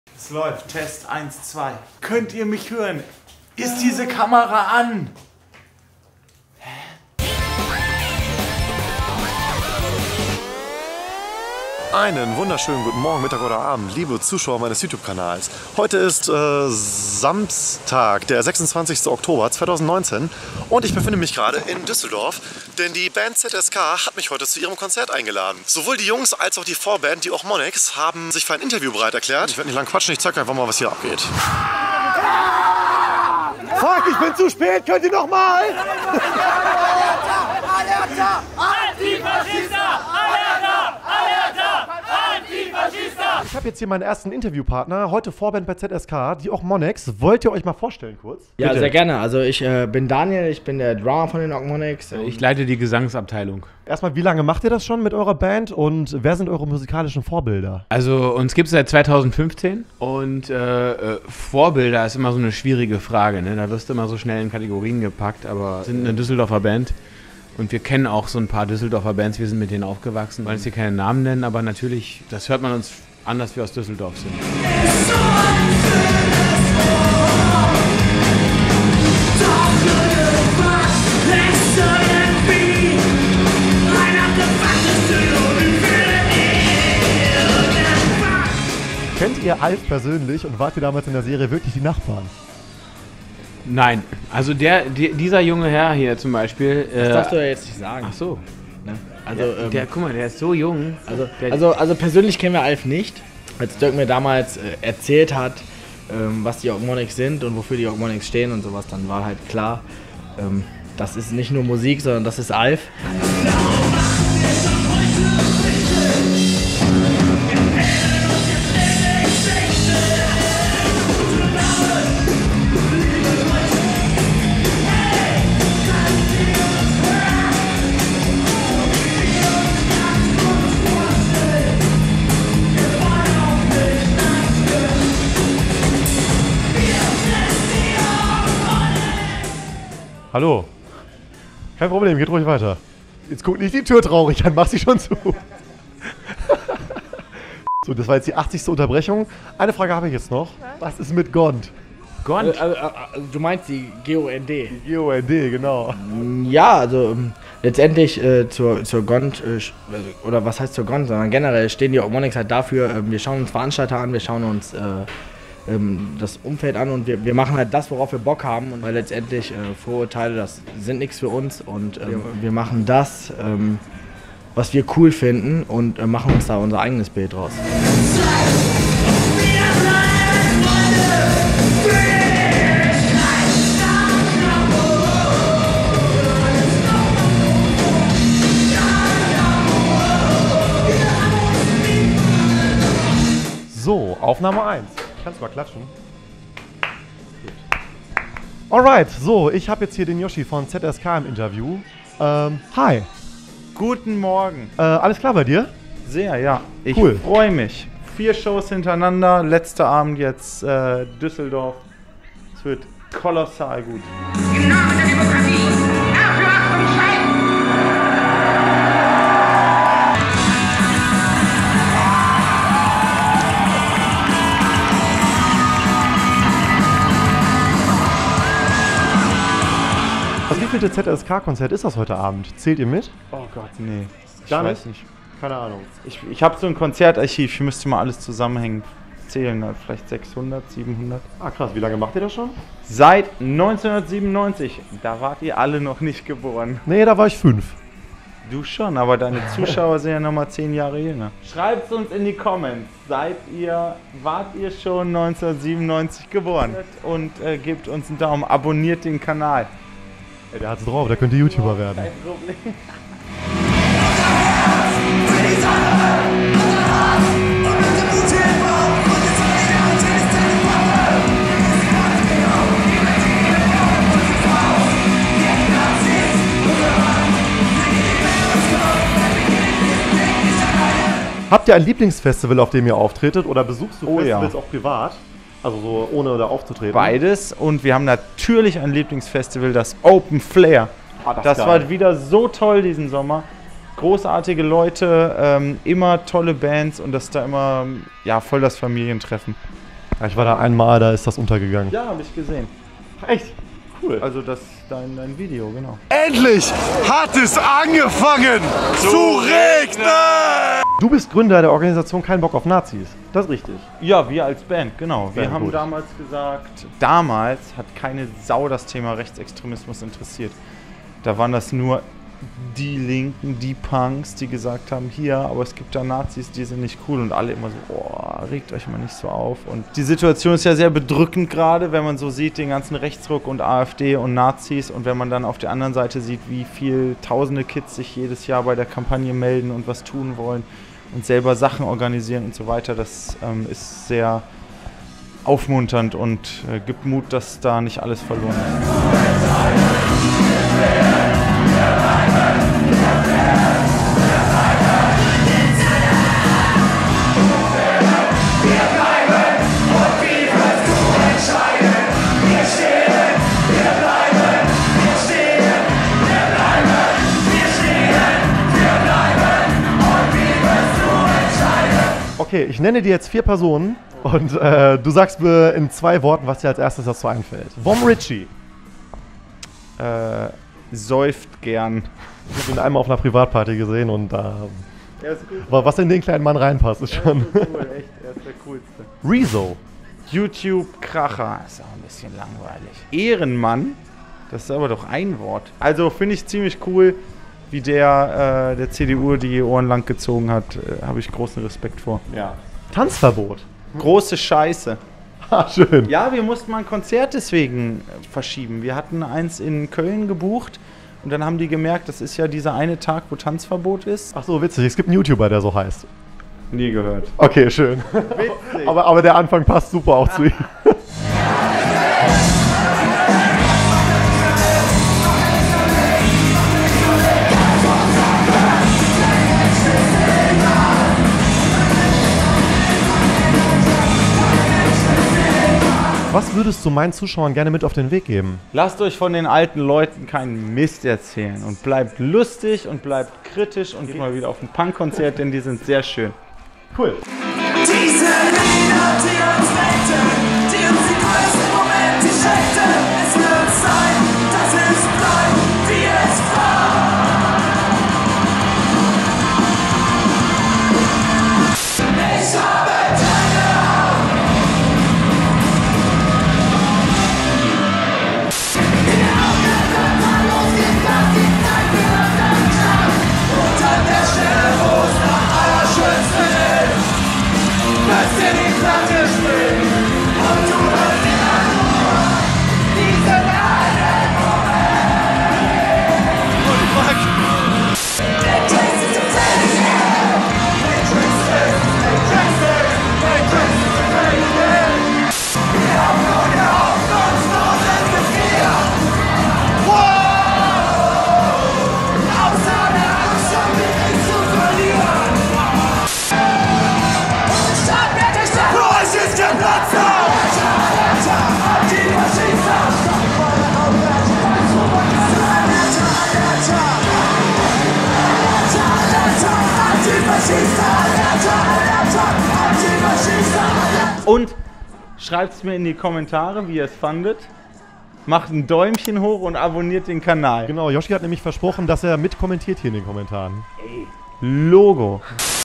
ZSK KonzertVlog + Interview über Nazis, Tour & Rezo
Hey! :) Hier mal ein Versuch, einen Konzertvlog in Verbindung mit einem Interview! Wie findet ihr es? Verbesserungsvorschläge?